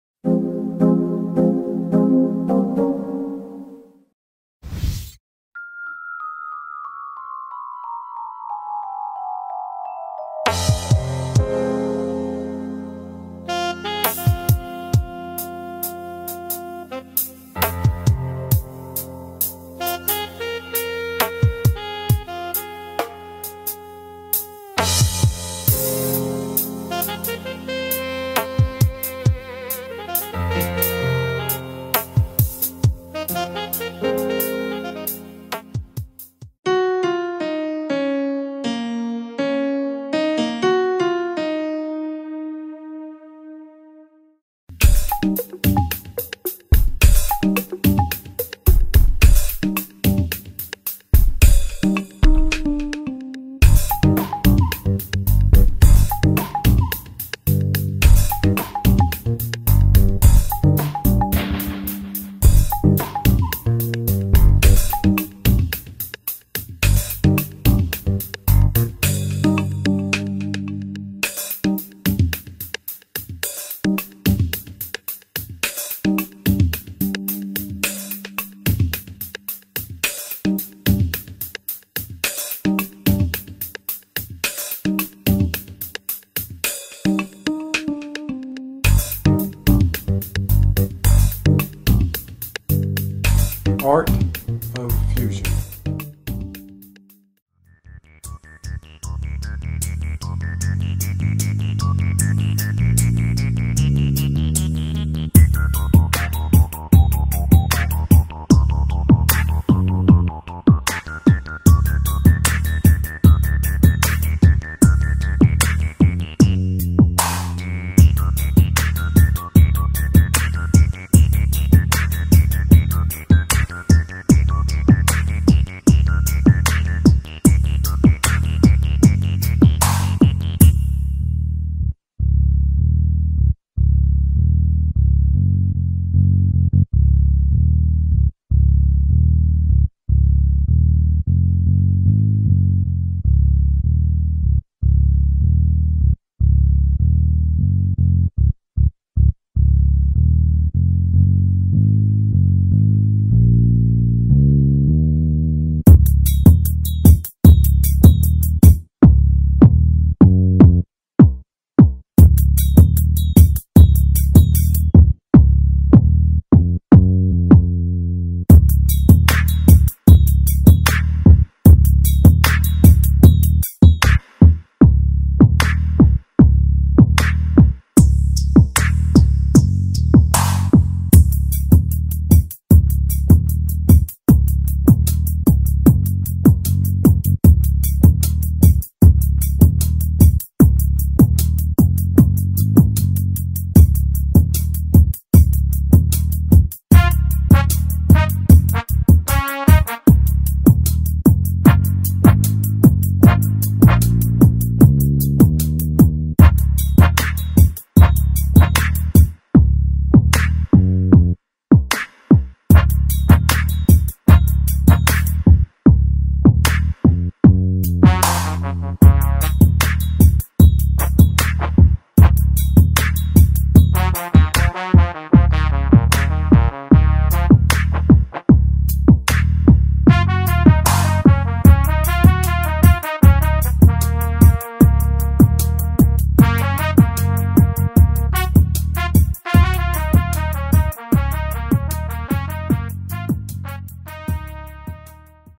Three at One Time-The Art of Fusion
Art and music fused to produce THREE paintings in the moment...Pure energy!